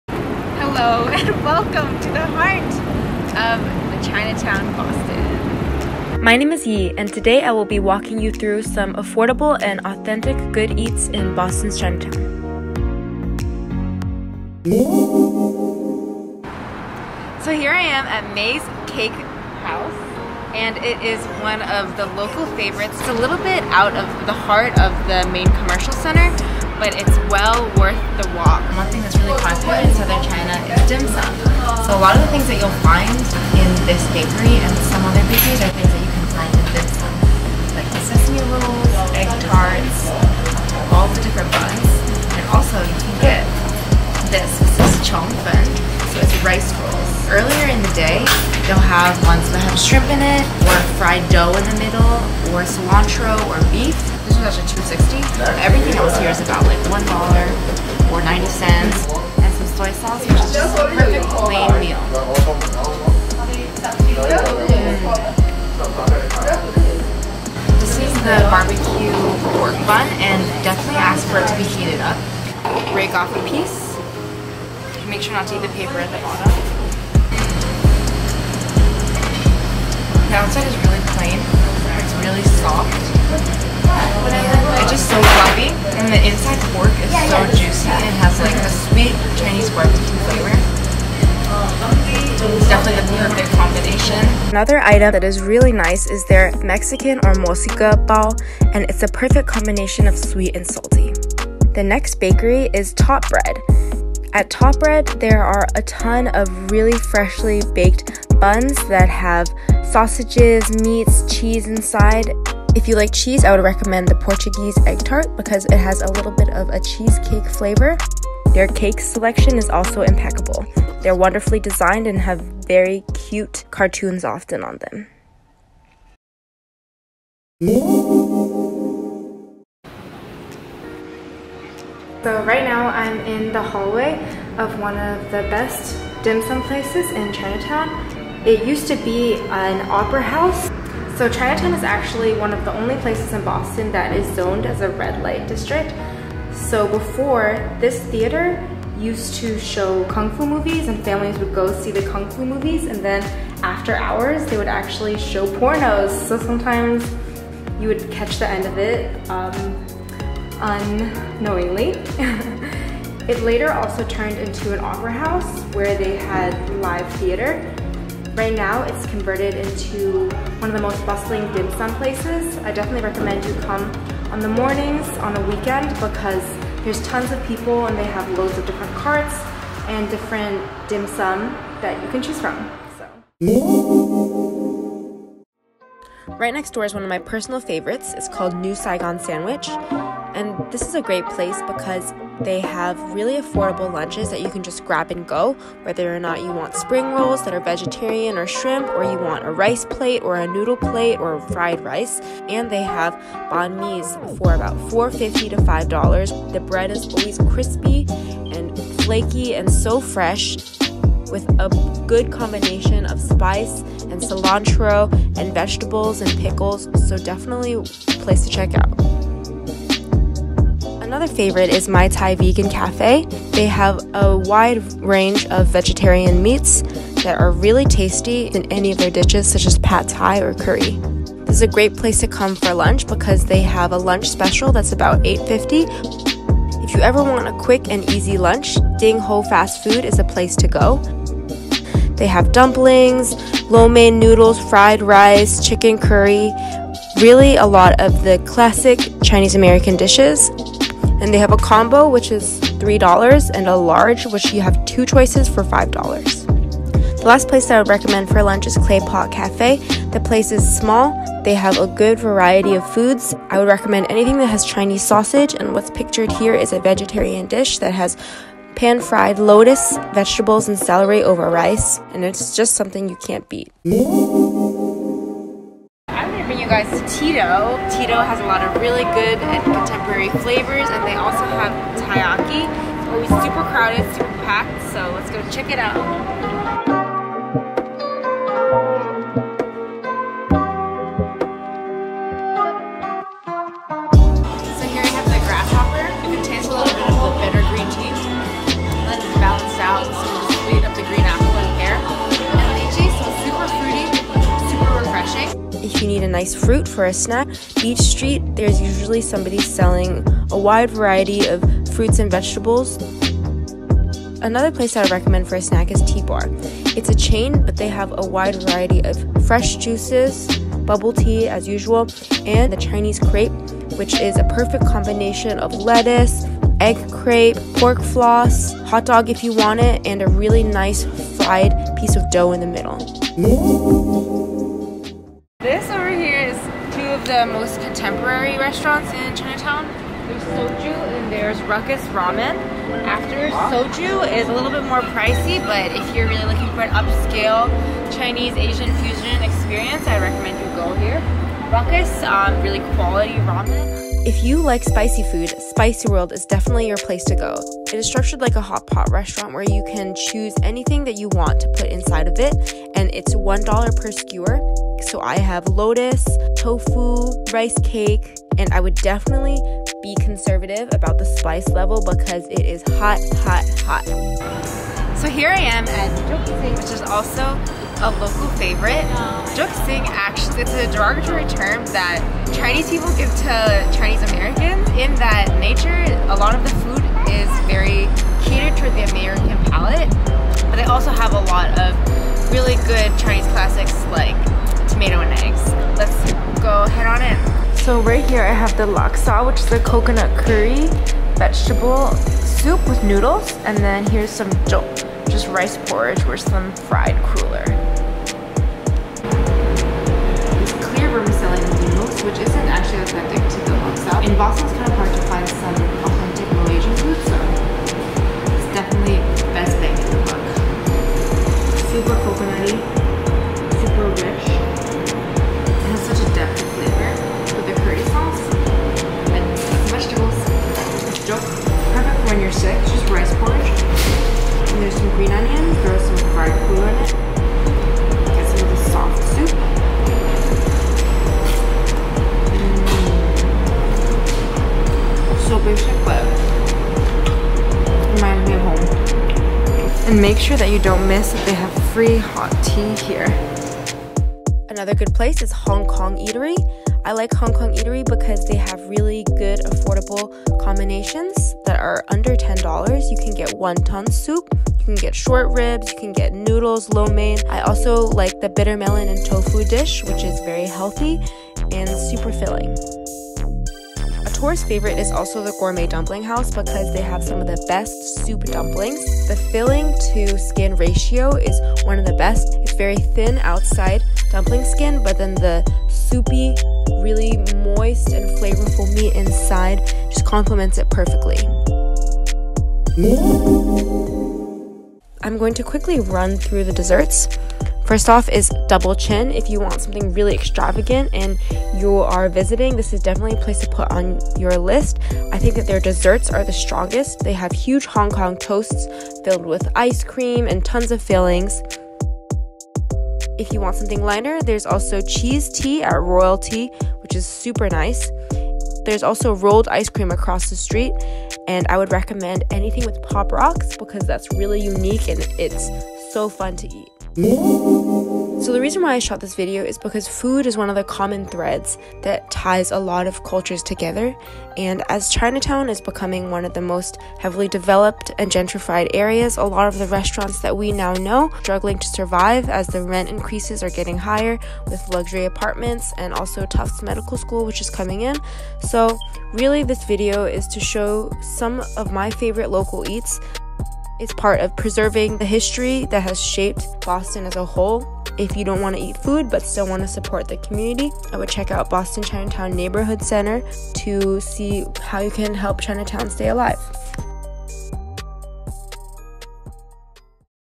LOCAL CHEAP EATS IN CHINATOWN, BOSTON (from an ~authentic local~) 波士顿唐人街
If you are visiting Boston or looking for something to eat and relatively cheap in Chinatown, this is the video for you!

If you have $1-10, I'd go to:
May's cake house (0:17)
Saigon Sandwich (3:05) or Ding Ho Fast Food (3:58)
Claypot Cafe (4:18) or Jook Sing (6:16)

If you wanna spend $5-15 per person
Empire Garden Dim Sum (2:10)
My Thai Vegan (3:41)
Spicy World (5:56)
Hong Kong Eatery (7:34)
Gourmet Dumpling House (7:54)

Snacks ($3-8)
Top Bread (1:41)
Tea-Do (4:38)
T-Baar (5:16)
RoyalTea (8:34)

If you tryin to splash out:
Shojo or Ruckus ramen (5:43)
Double Chin (8:16)

Boston's Chinatown is definitely underrated but there are a lot of hidden gems!

Also, apologies for the discrepancies in audio, and thanks to Daniela for helping to film!

X yi